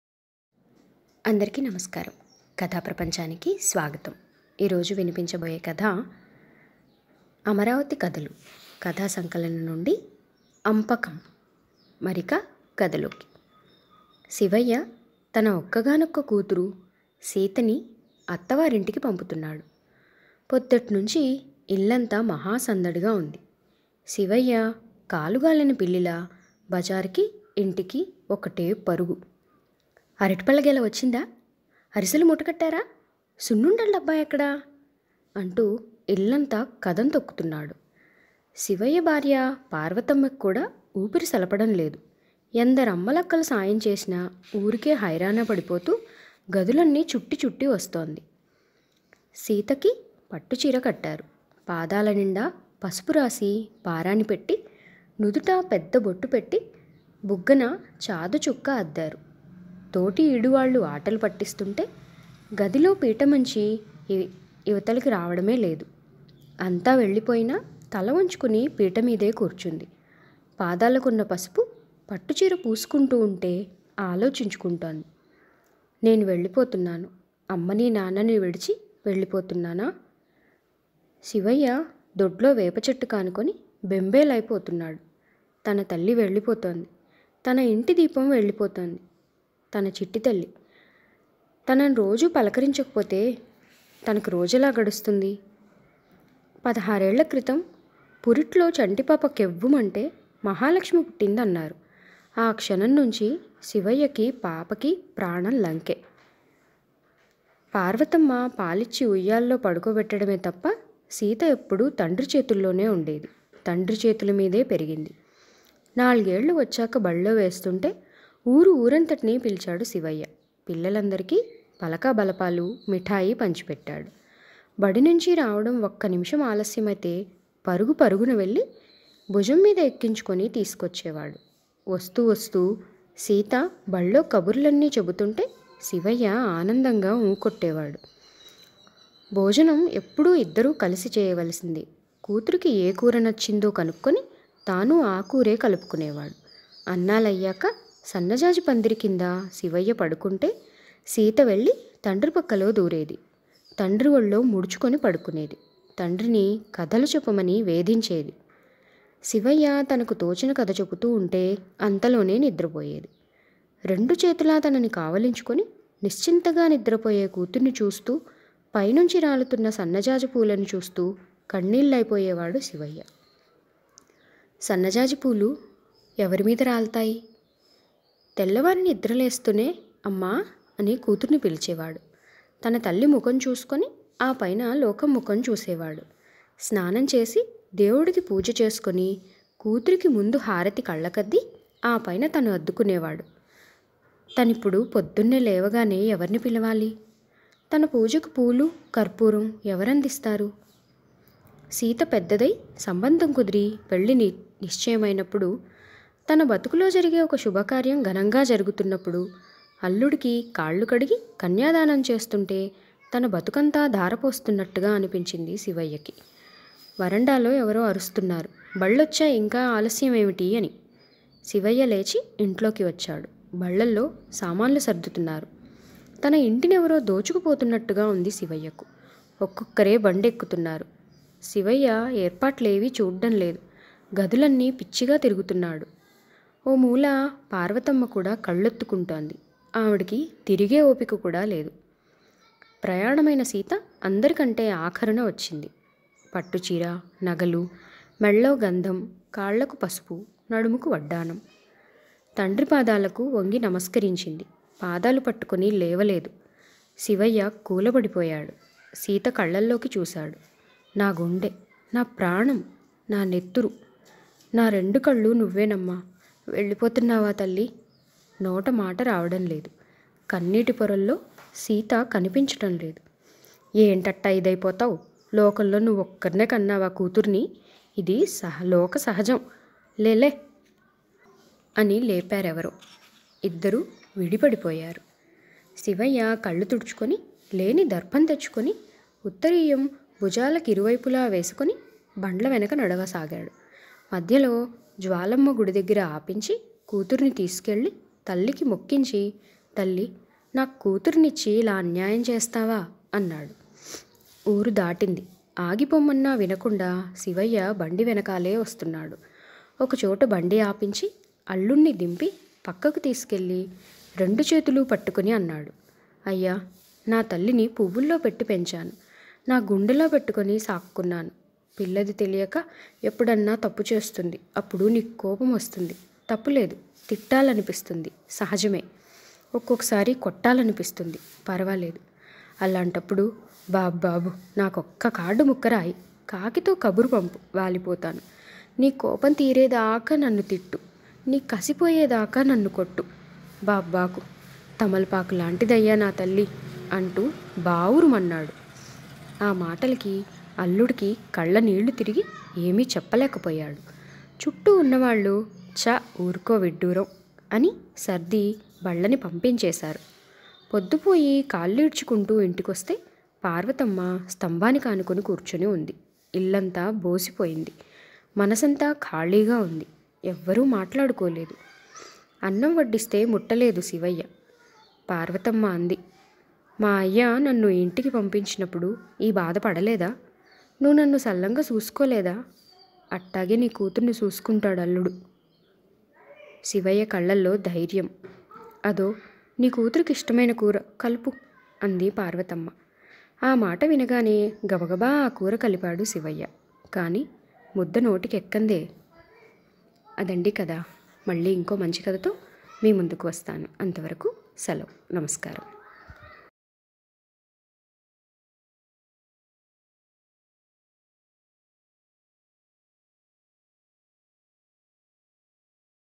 కూతురంటే ప్రాణం శివయ్య కి!కాళ్ళు కడిగి కన్యాదానం చేస్తుంటే తన బతుకంతా ధారపొస్తున్నట్టు అనిపించింది..
ఏంటట్ట ఇదైపోతావ్!లోకంలో నువ్వే కన్నావా... కూతుర్ని!!